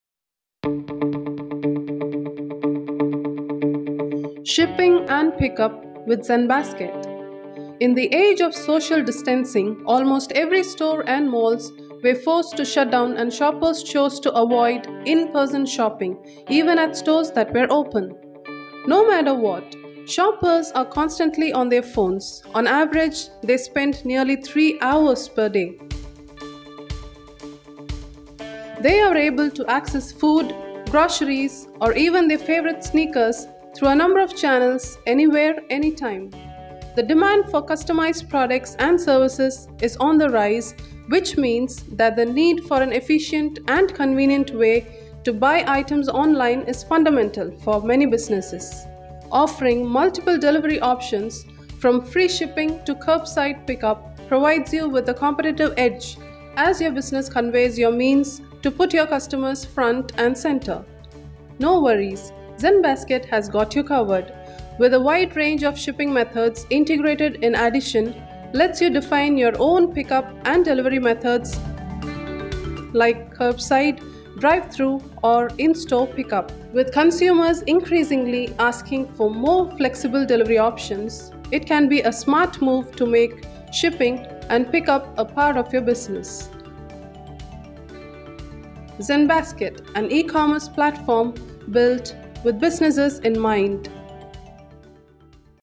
Zenbasket shipping & in store pickup
With consumers increasingly asking for more flexible delivery options, it can be a smart move to make shipping & pickup a part of your business.